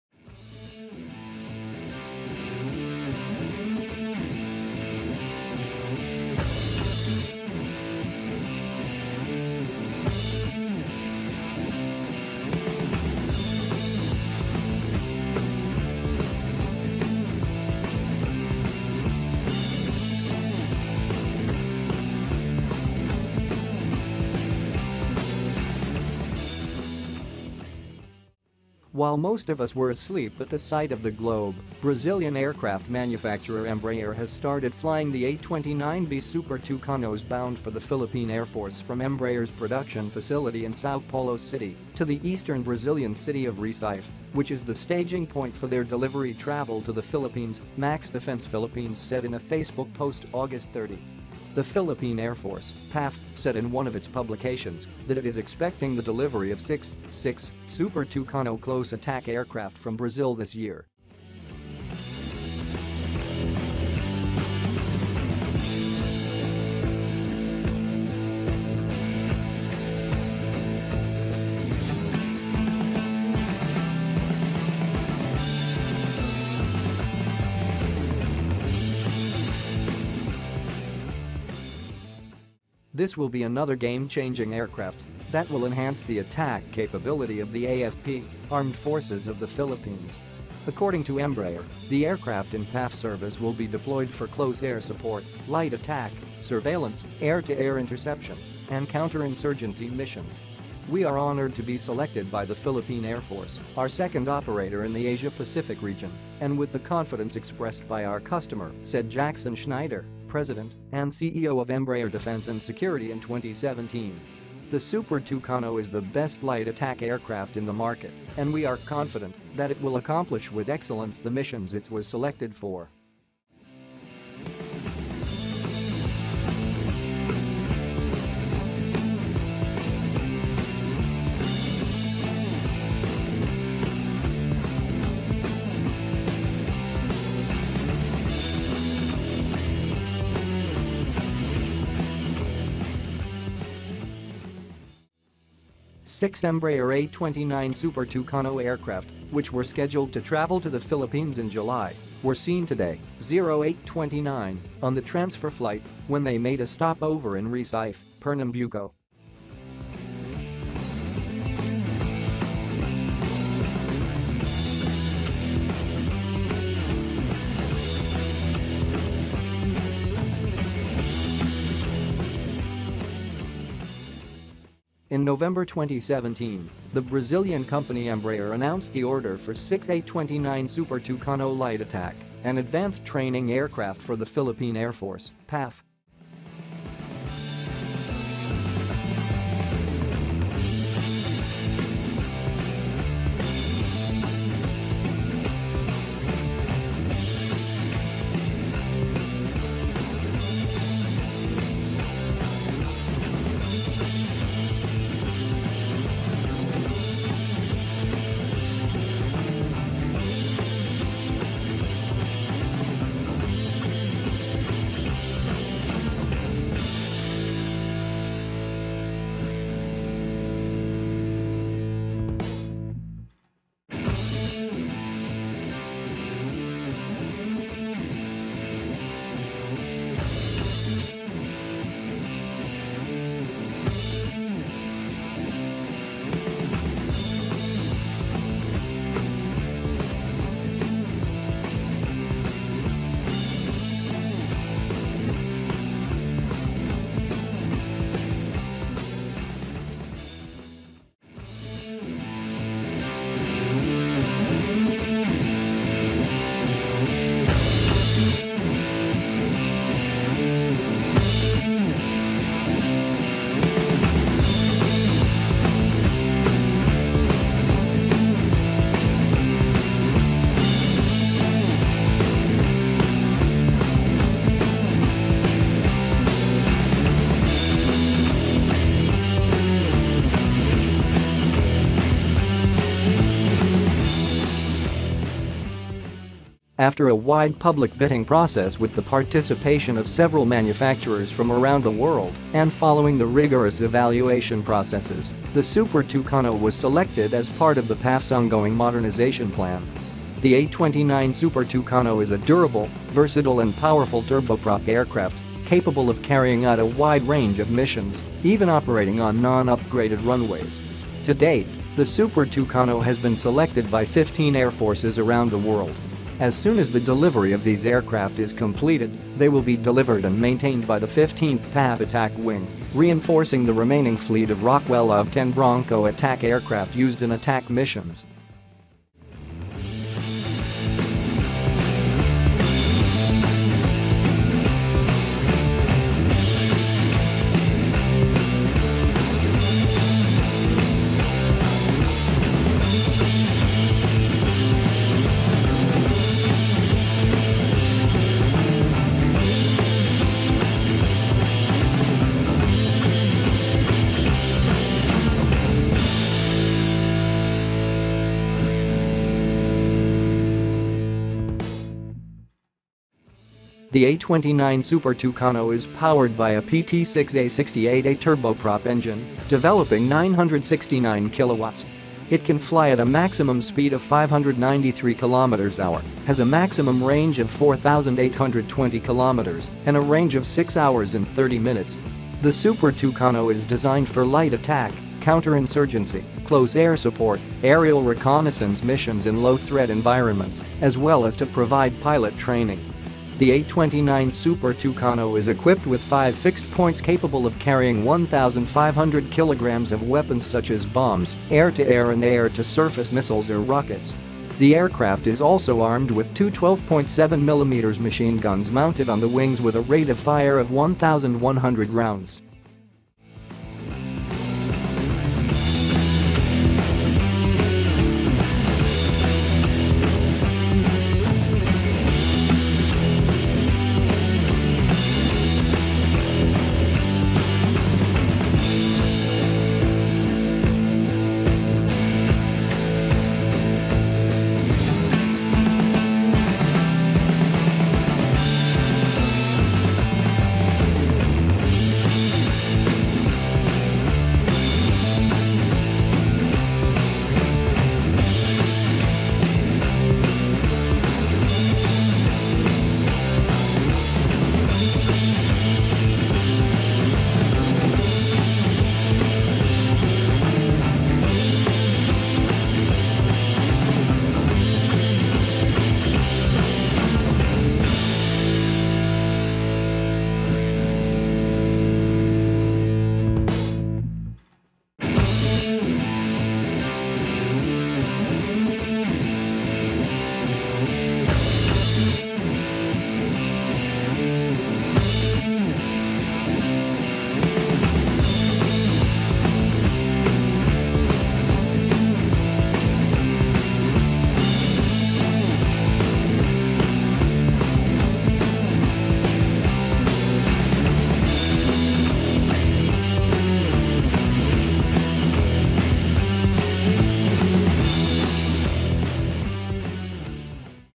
FINALLY! Six Embraer A-29 Super Tucano aircraft from brazil for PAF will arrive today!
“While most of us were asleep at this side of the globe, Brazilian aircraft manufacturer Embraer has started flying the A-29B Super Tucanos bound for the Philippine Air Force from Embraer’s production facility in Sao Paulo City, to the eastern Brazilian city of Recife, which is the staging point for their delivery travel to the Philippines,” MaxDefense Philippines said in a Facebook post August 30.

The Philippine Air Force (PAF) said in one of its publications that it is “expecting the delivery of six (6) Super Tucano Close Attack Aircraft from Brazil this year. This will be another game changing aircraft that will enhance the attack capability of the AFP (Armed Forces of the Philippines).”